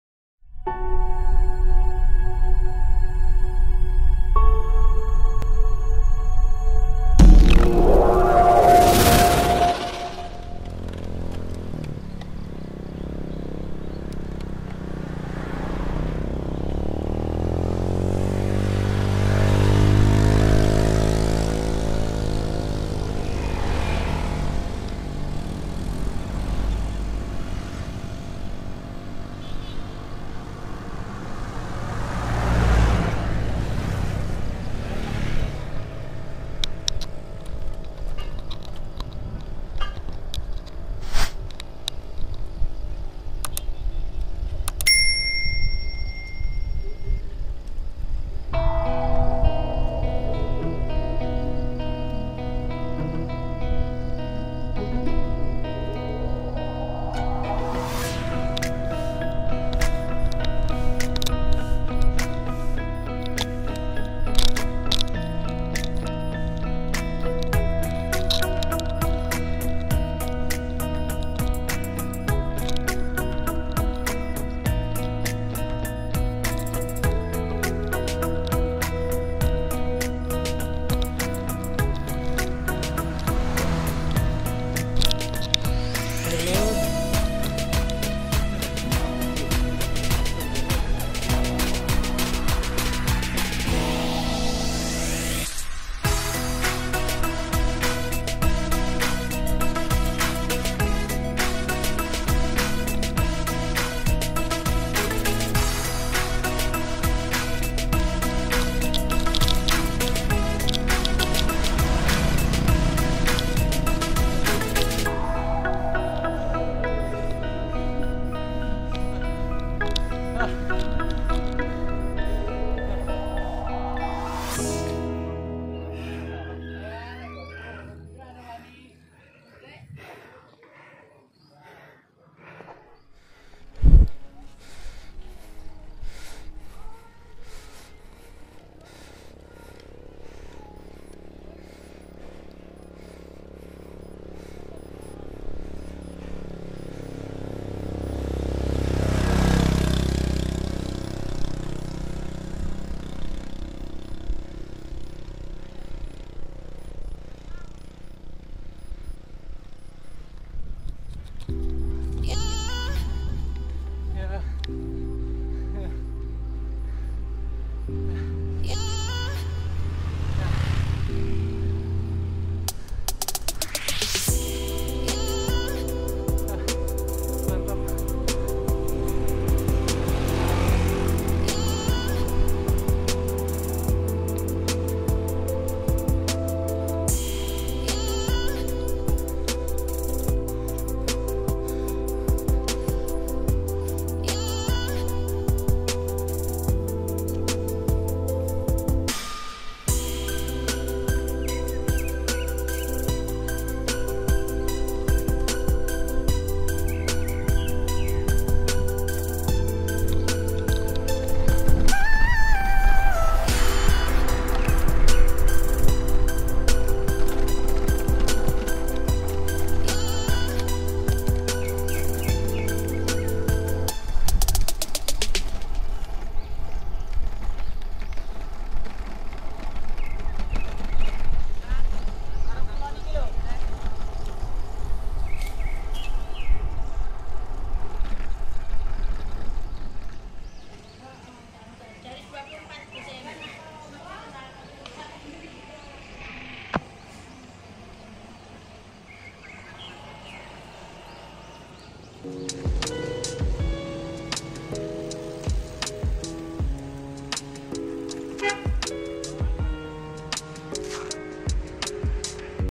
GOWES OBELIX HILLS JOGJA | Roadbike Indonesia
divideo kali ini perjalanan bersepeda ke obelix hills via Breksi, candi ijo